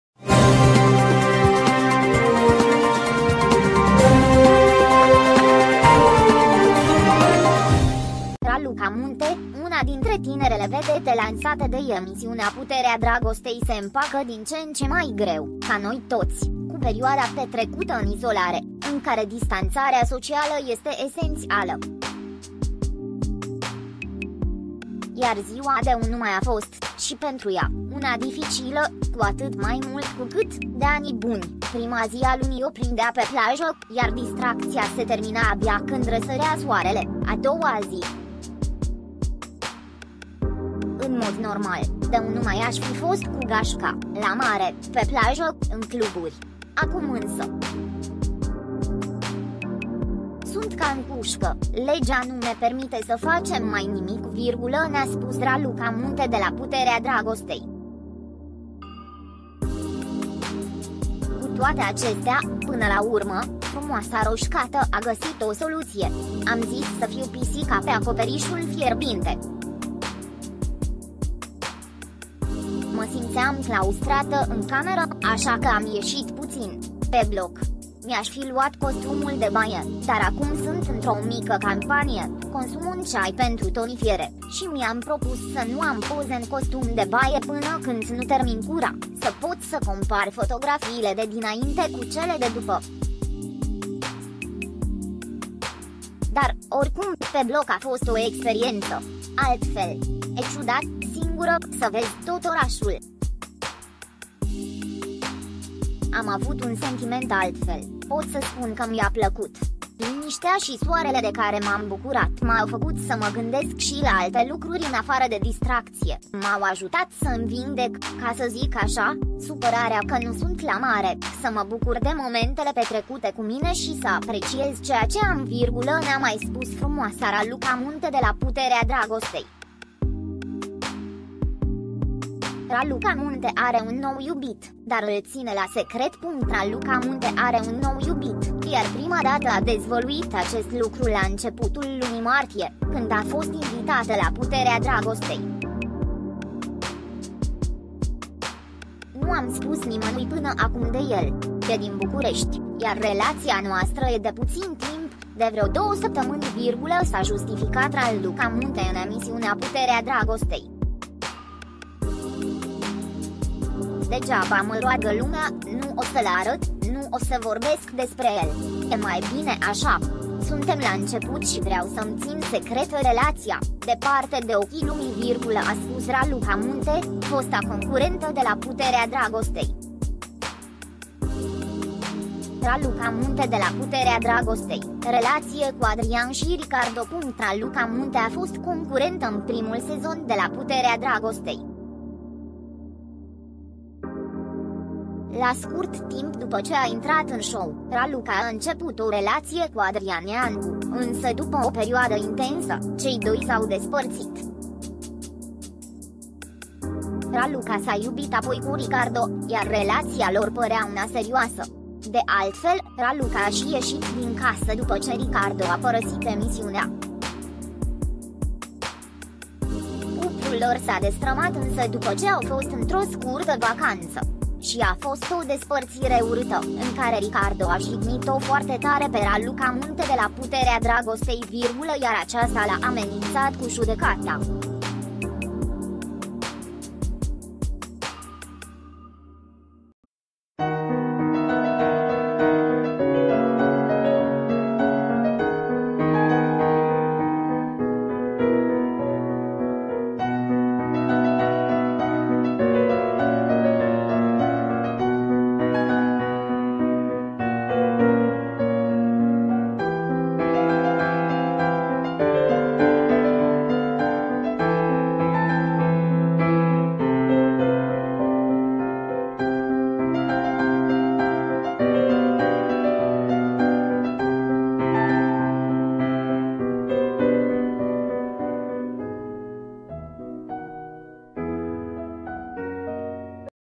Raluca Munte de la ”Puterea Dragostei”, show pe bloc, de 1 Mai! ”Am zis să fiu pisica pe acoperișul
Raluca Munte, una dintre tinerele vedete lansate de emisiunea ”Puterea Dragostei” se împacă din ce în ce mai greu, ca noi toți, cu perioada petrecută în izolare, în care distanțarea socială este esențială.

Iar ziua de 1 Mai a fost , și pentru ea, una dificilă, cu atât mai mult cu cât, de ani buni, prima zi a lunii o prindea pe plajă, iar distracția se termina abia când răsărea soarele, a doua zi.

”În mod normal, de 1 Mai aș fi fost cu gașca, la mare, pe plajă, în cluburi. Acum însă... sunt ca în cușcă, legea nu ne permite să facem mai nimic”, ne-a spus Raluca Munte de la ”Puterea Dragostei”.

Cu toate acestea, până la urmă, frumoasa roșcată a găsit o soluție. ”Am zis să fiu pisica pe acoperișul fierbinte. Mă simțeam claustrată în cameră, așa că am ieșit puțin... pe bloc. Mi-aș fi luat costumul de baie, dar acum sunt într-o mică campanie, consum un ceai pentru tonifiere, și mi-am propus să nu am poze în costum de baie până când nu termin cura, să pot să compar fotografiile de dinainte cu cele de după. Dar, oricum, pe bloc a fost o experiență... altfel.  E ciudat, singură, să vezi tot orașul. Am avut un sentiment altfel, pot să spun că mi-a plăcut. Liniștea și soarele de care m-am bucurat m-au fcut să mă gândesc și la alte lucruri în afară de distracție, m-au ajutat să îmi vindec, ca să zic așa, supărarea că nu sunt la mare, să mă bucur de momentele petrecute cu mine și să apreciez ceea ce am”, ne-a mai spus frumoasa Raluca Munte de la ”Puterea Dragostei”.

Raluca Munte are un nou iubit, dar îl ține ”la secret”
Raluca Munte are un nou iubit, iar prima dată a dezvăluit acest lucru la începutul lunii martie, când a fost invitată la Puterea Dragostei. ”Nu am spus nimănui până acum de el. E din București, iar relația noastră e de puțin timp, de vreo două săptămâni”, s-a justificat Raluca Munte în emisiunea Puterea Dragostei. ”Degeaba mă roagă lumea, nu o să-l arăt, nu o să vorbesc despre el. E mai bine așa! Suntem la început și vreau să-mi țin secretă relația, departe de ochii lumii”, a spus Raluca Munte, fosta concurentă de la Puterea Dragostei.

Raluca Munte de la Puterea Dragostei, relație cu Adrian și Ricardo
Raluca Munte a fost concurentă în primul sezon de la Puterea Dragostei. La scurt timp după ce a intrat în show, Raluca a început o relație cu Adrian Iancu, însă după o perioadă intensă, cei doi s-au despărțit. Raluca s-a iubit apoi cu Ricardo, iar relația lor părea una serioasă. De altfel, Raluca a și ieșit din casă după ce Ricardo a părăsit emisiunea. Cuplul lor s-a destrămat însă după ce au fost într-o scurtă vacanță. Și a fost o despărțire urâtă, în care Ricardo a jignit-o foarte tare pe Raluca Munte de la ”Puterea Dragostei”, iar aceasta l-a amenințat cu judecata.